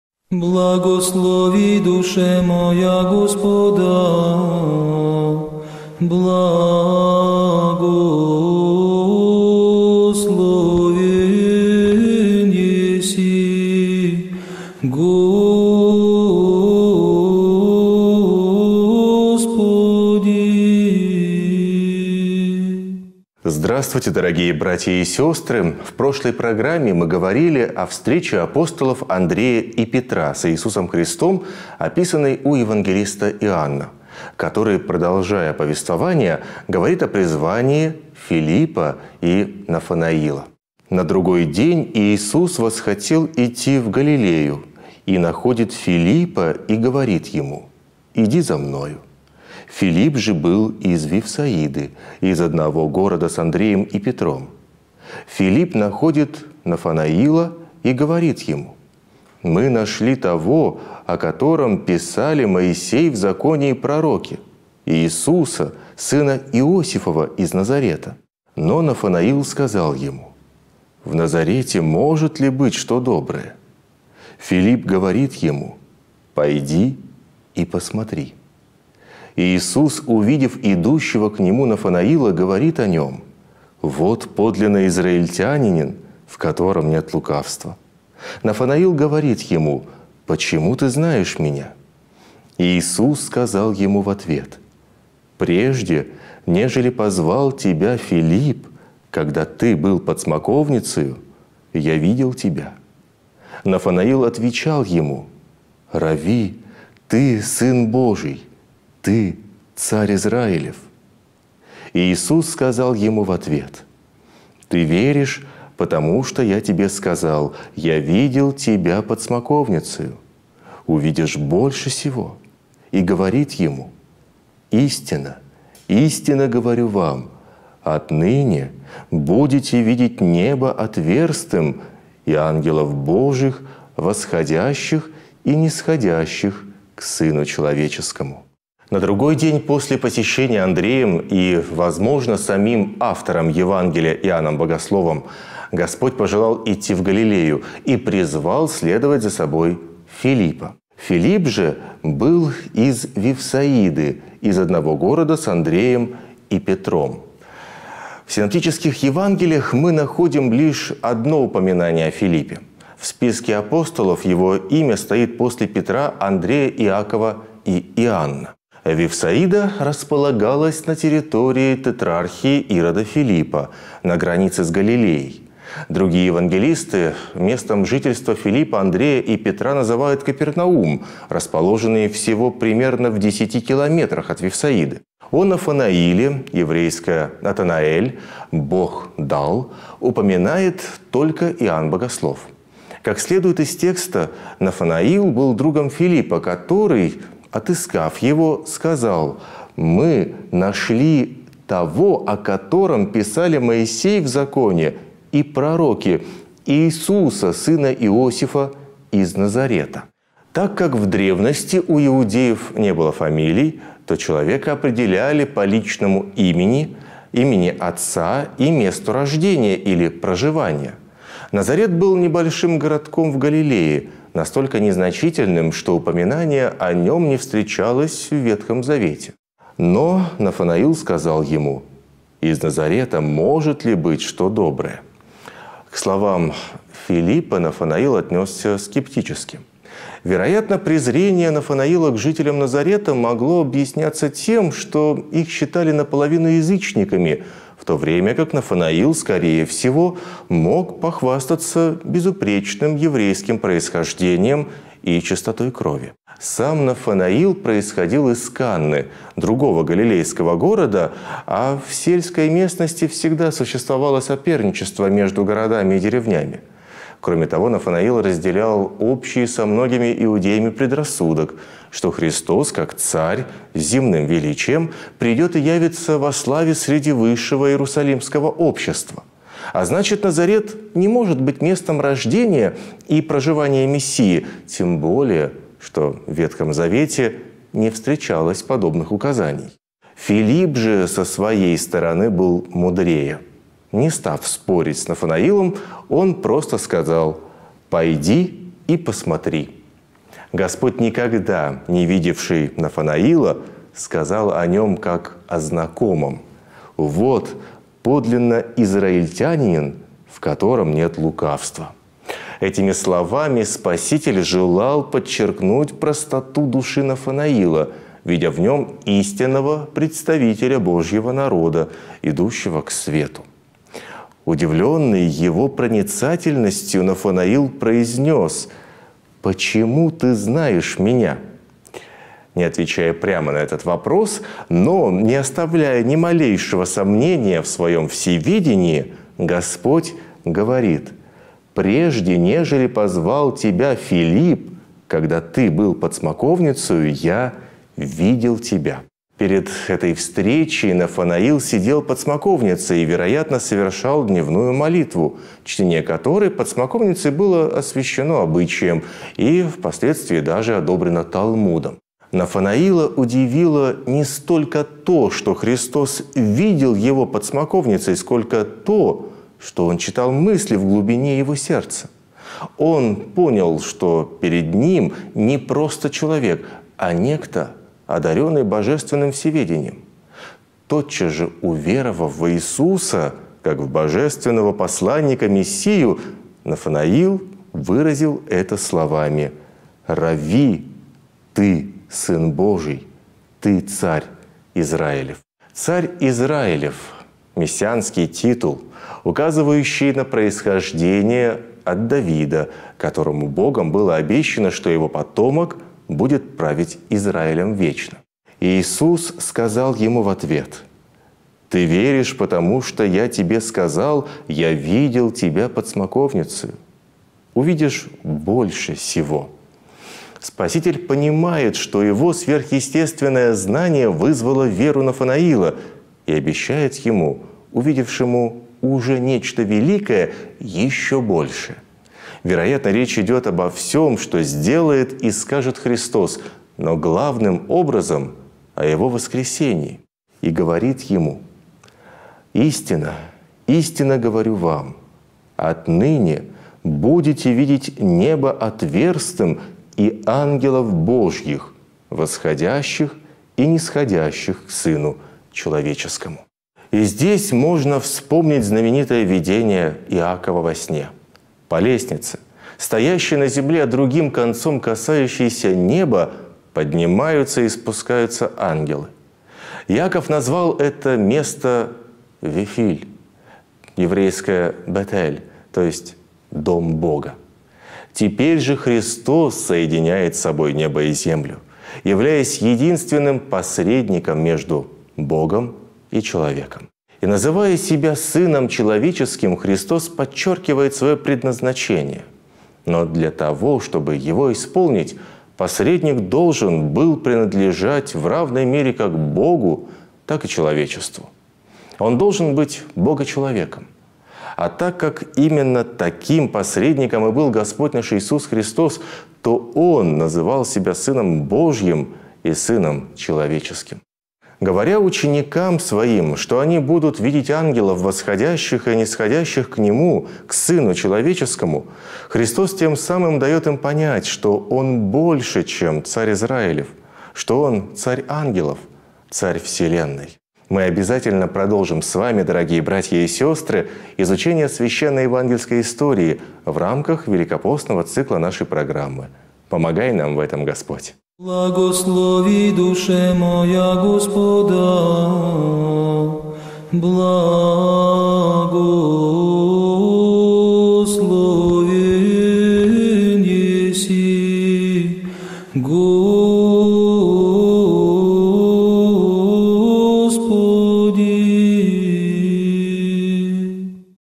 Читаем Евангелие 12 апреля 2021. Евангелие от Иоанна. Глава 1, ст. 43-51.
Нафанаил отвечал Ему: Равви́! Ты Сын Божий, Ты Царь Израилев.